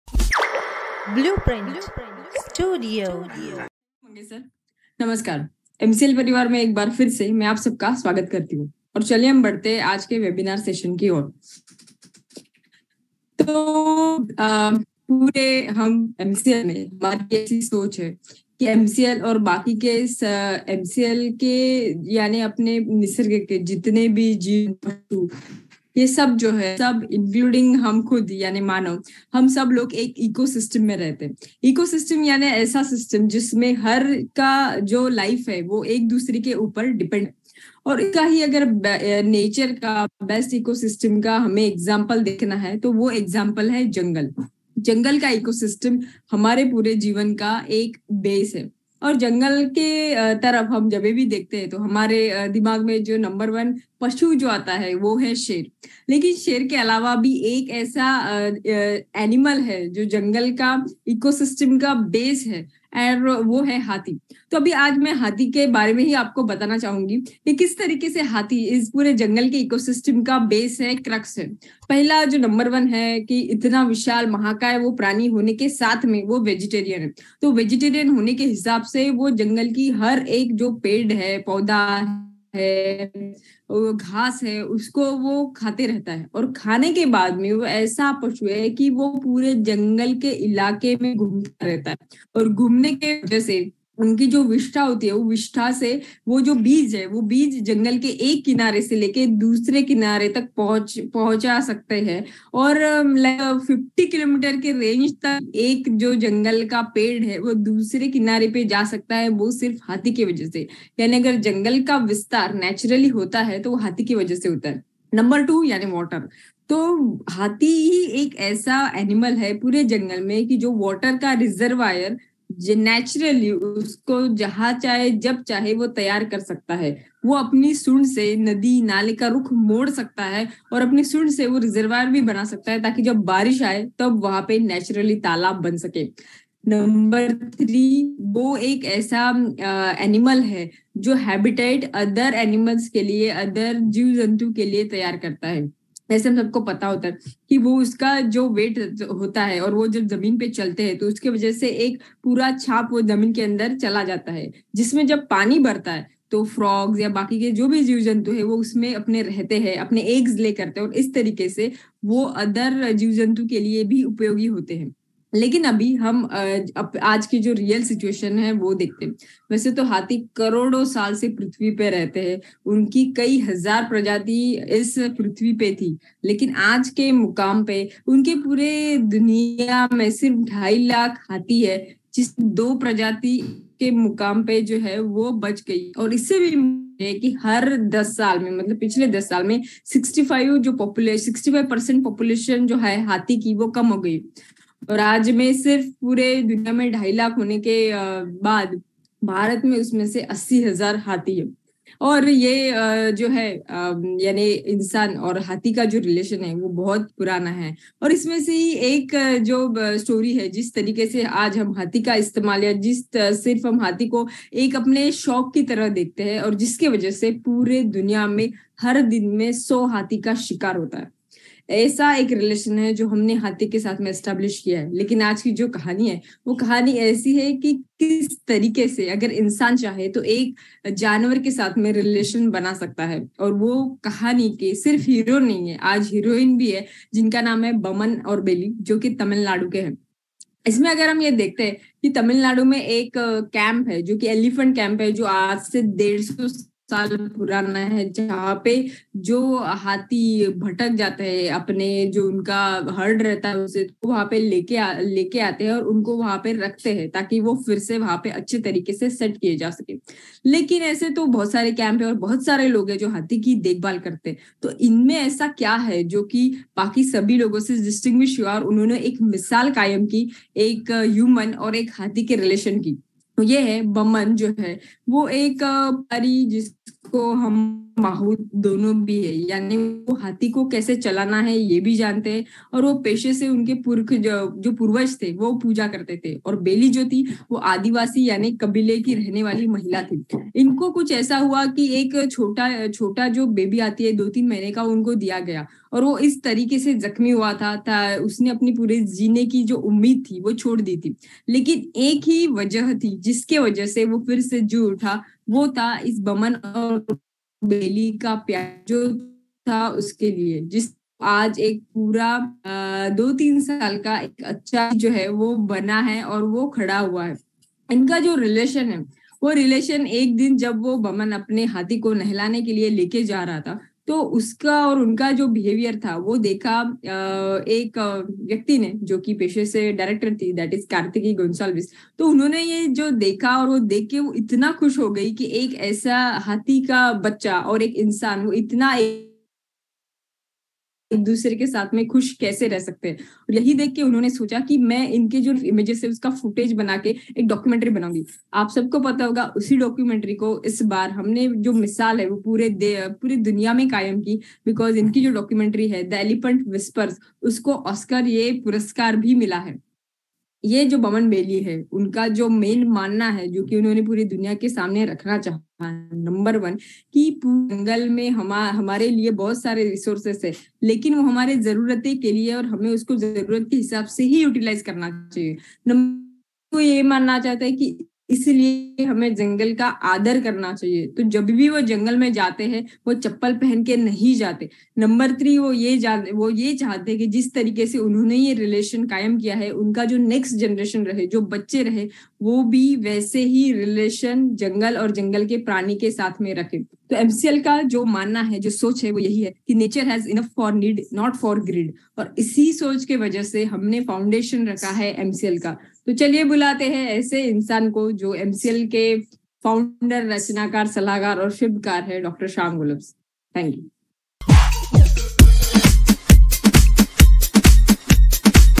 Revolution Begins - Episode 31 - PRACHI MAM - ( Session by DR Sham Gholap Sir)
Greetings from Valsad Cleanfuels LTD !

This Playlist is made for the MCL - MBS owners, who couldn't  attend the Friday session as they miss the MEETING.

In general these sessions are not open for all, but as Dr, Sham Gholap sir talk about "New Bharat" and its upcoming Fuel Industry. This session is beneficial for all who dream of - "SUJALAM SUFALAM BHARAT"